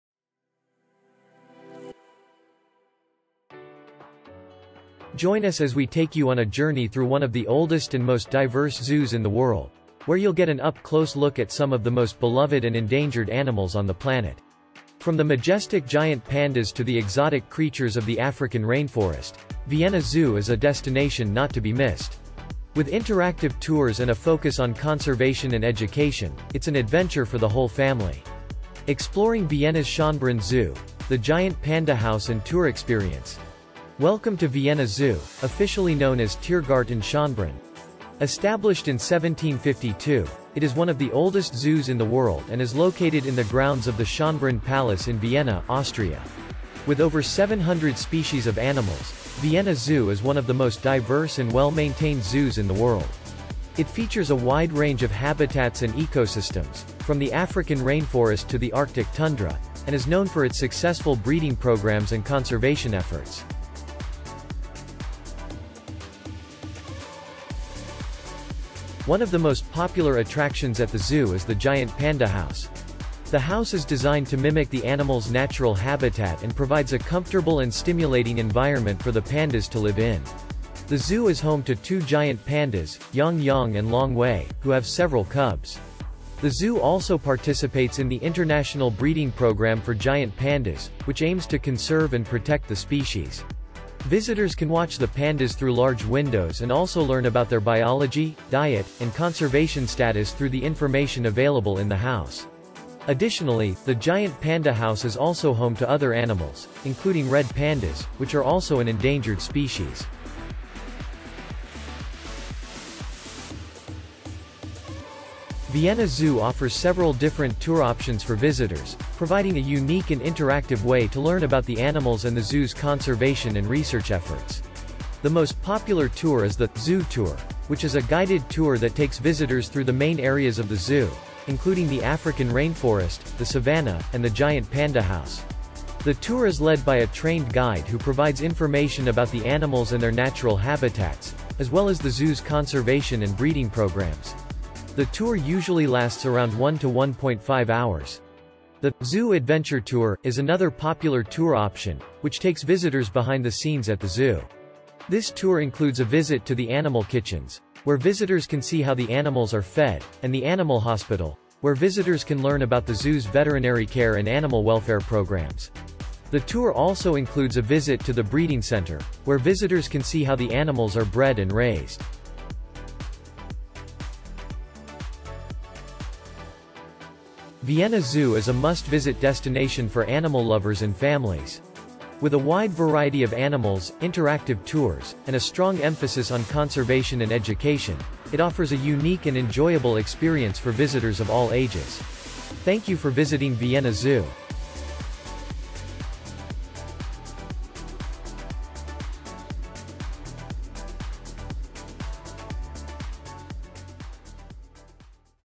Exploring Vienna's Schönbrunn Zoo: The Giant Panda House and Tour Experience?
In this Vienna zoo video, we'll explore the Giant Panda House and Tour Experience. This is the oldest zoo in Vienna and home to some of the zoo's most famous animals, including the giant panda.

If you're visiting Vienna and looking for a fun activity to do, be sure to check out the Giant Panda House and Tour Experience! This tour is perfect for those who are interested in animal conservation and want to learn more about Vienna's history. You'll get to see some of the zoo's most beloved animals up close and learn about their conservation efforts.

Video Chapters:
0:00  Introduction
1:05  The Giant Panda House: A Closer Look
1:51  Get a Behind-the-Scenes Look with the Interactive Tour
2:54  Conservation & Education at Vienna's Schönbrunn Zoo